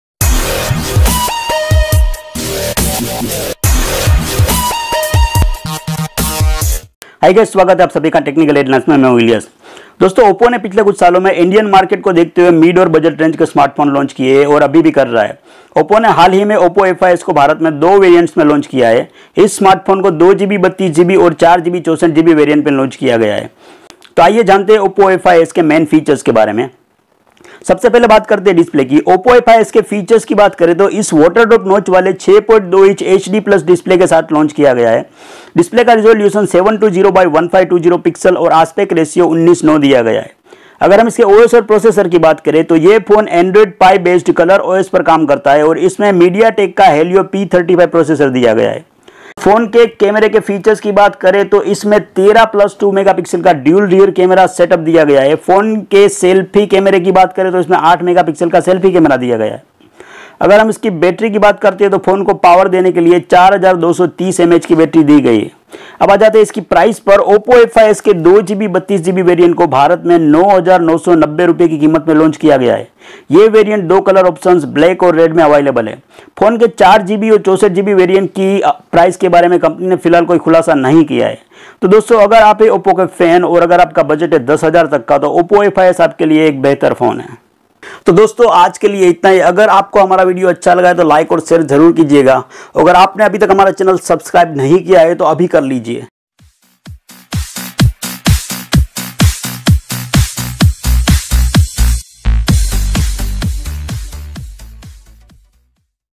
Oppo A5s specifications, price and features
Hi Guys
In this video we have given information about oppo's new model Oppo A5s. Oppo A5s is one of the best midrange phone in oppo mobile series. It can give good competition to redmi 7 and redmi note 7.

TECHNICAL HEADLINES